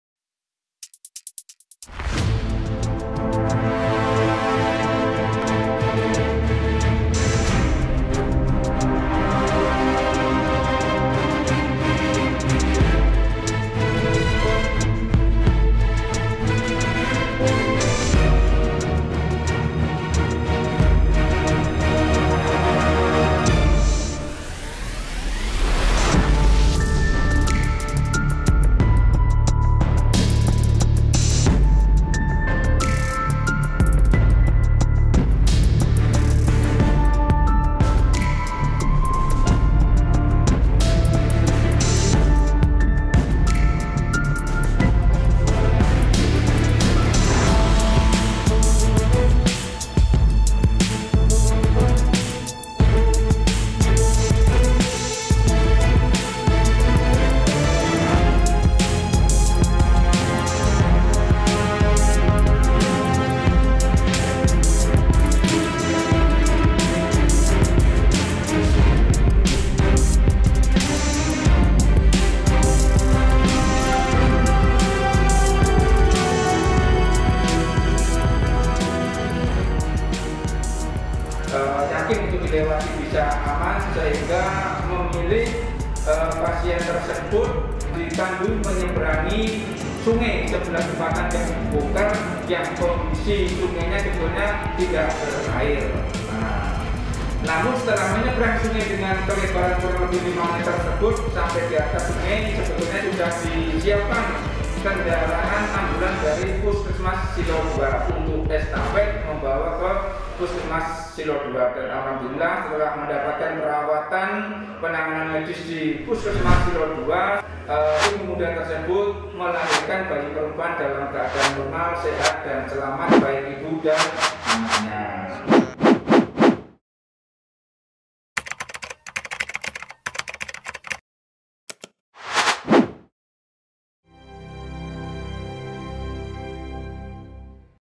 Detik-detik Ibu Hamil Ditandu ke Puskesmas
Seorang ibu yang hendak melahirkan di Kecamatan Silo, Jember, Jawa Timur, terpaksa ditandu melewati sungai kering di tengah perkebunan agar bisa segera tiba di puskesmas, Selasa 21 September 2021.